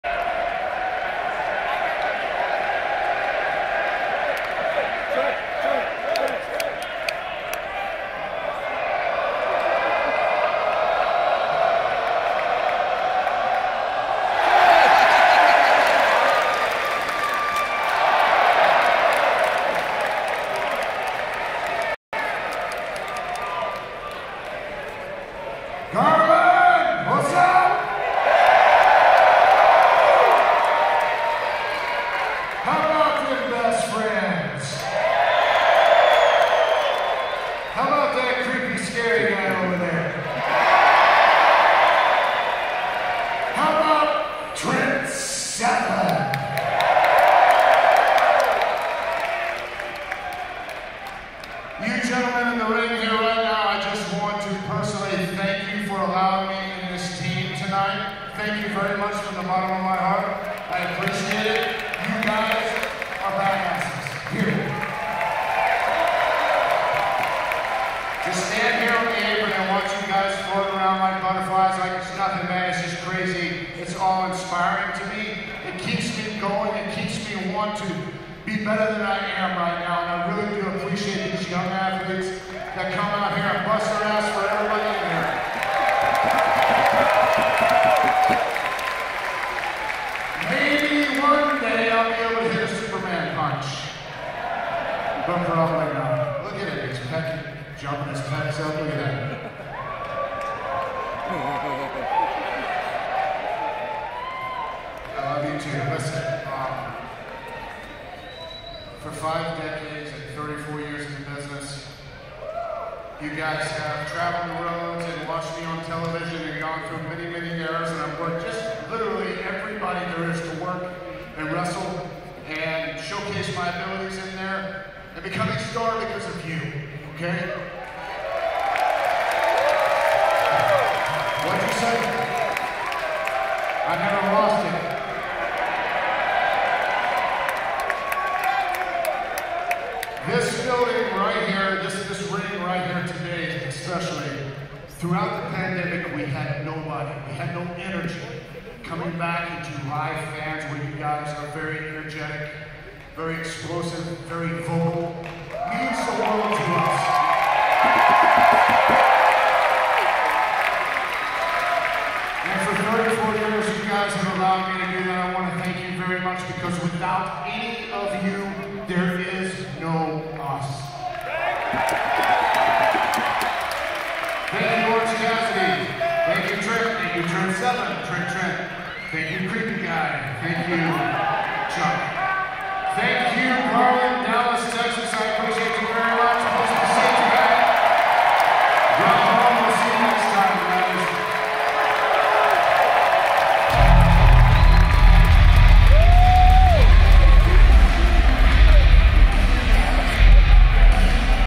Addressing the Crowd After Recording of December 16, 2022 AEW Rampage
Slight spoilers for the end of Rampage from Garland TX.
Sorry the quality is rough, I was zoomed all the way in.

Featuring: Dustin Rhodes, Orange Cassidy, Chuck Taylor, Trent Beretta, Trent Seven, and Danhausen